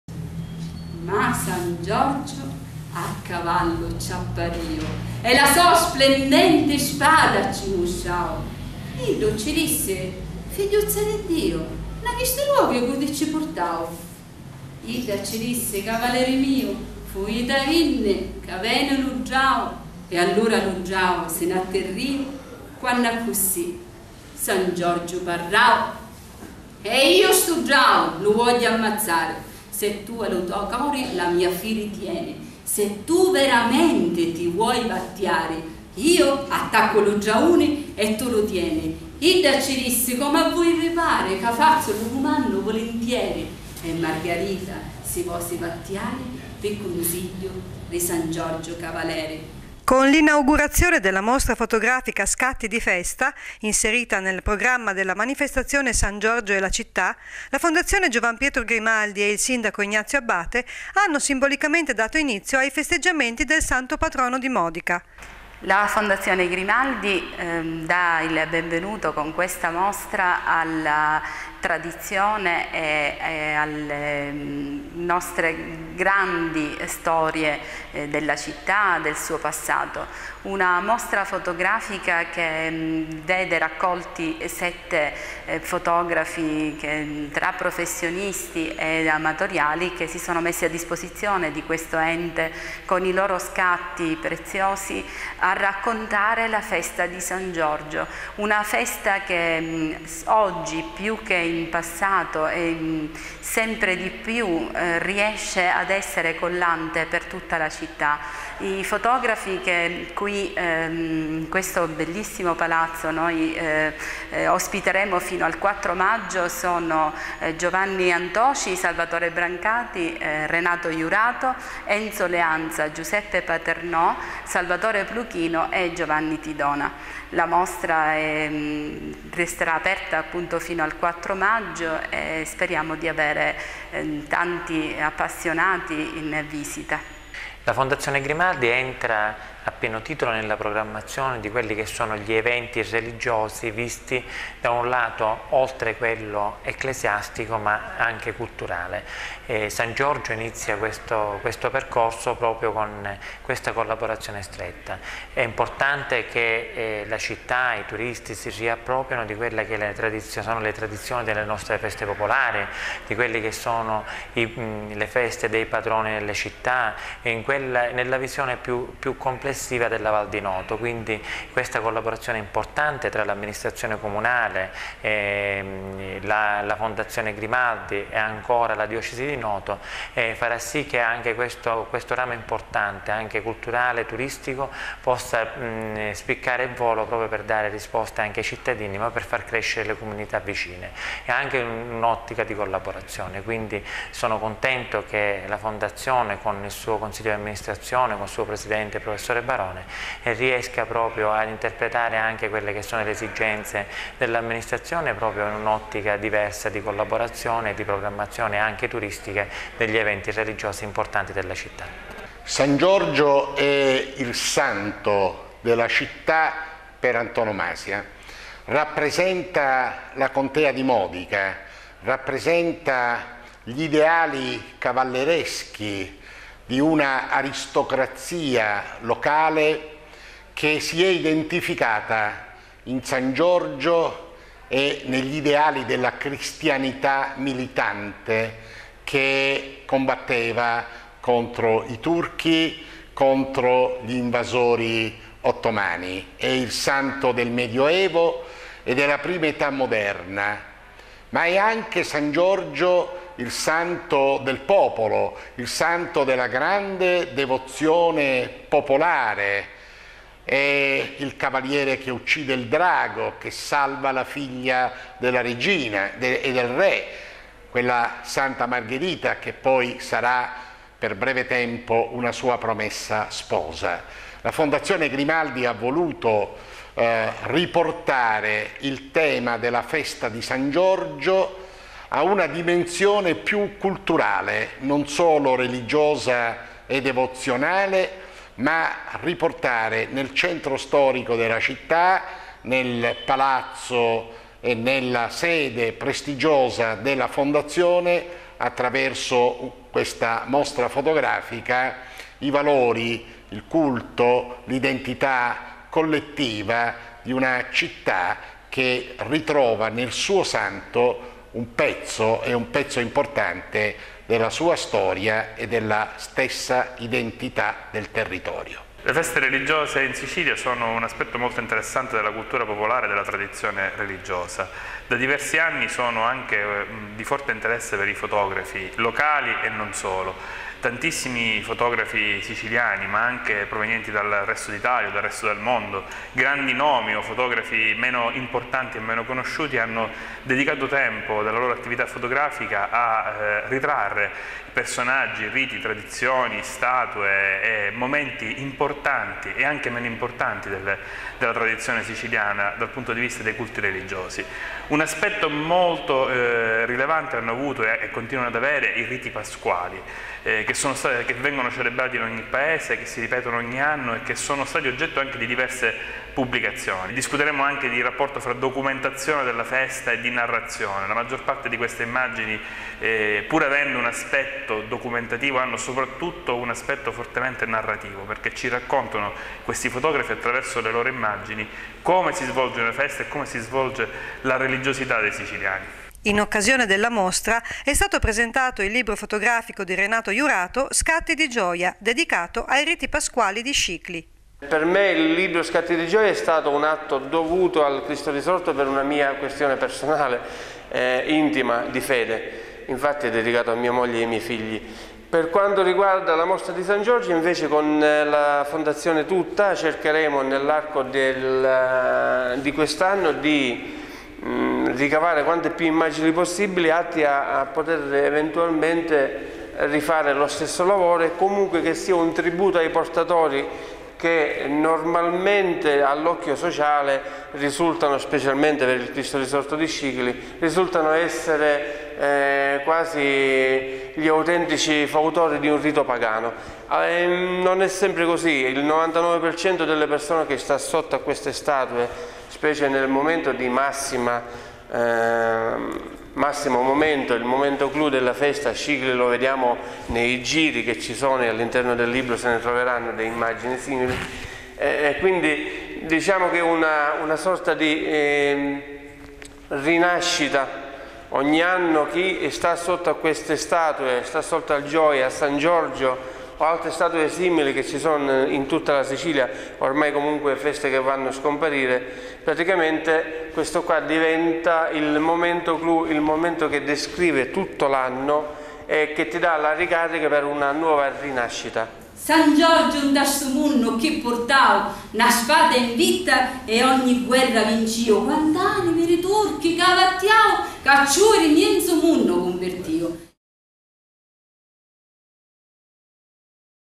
San Giorgio e la città - Mostra fotografica
La fondazione Grimaldi presenta: San Giorgio e la città - Mostra fotografica - Presentazione del libro "Scatti di gioia" di Renato Iurato